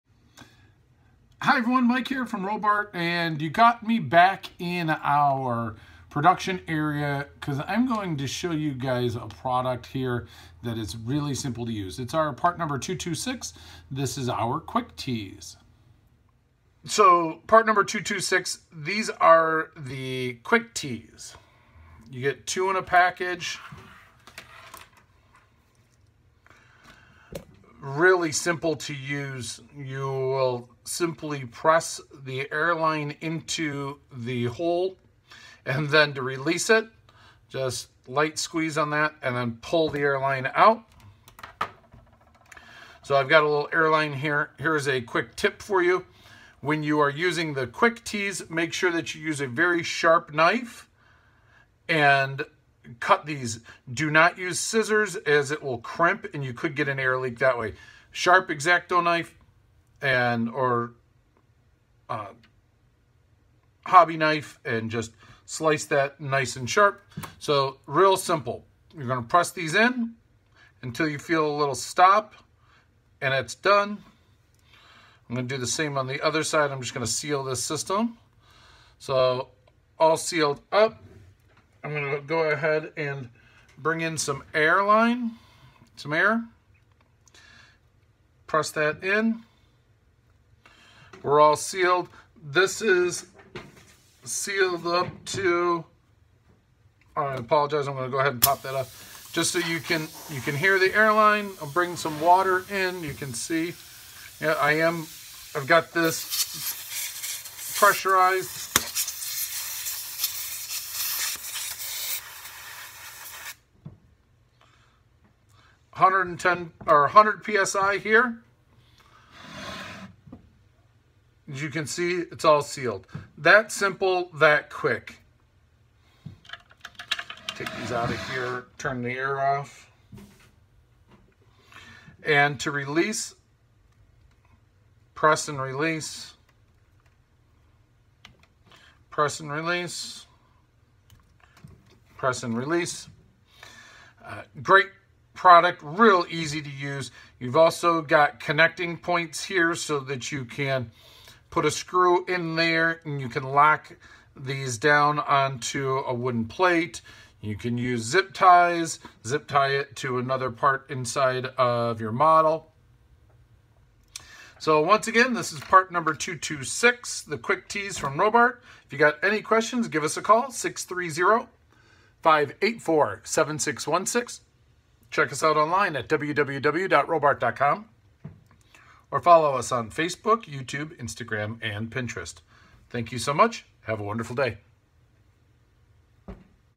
Robart Pneumatic QUICK TEES
Looking for a quick way to put your pneumatic system together?  Use the Robart Quick Tees (Part #226).  Simply push the airline into the tee and you are done!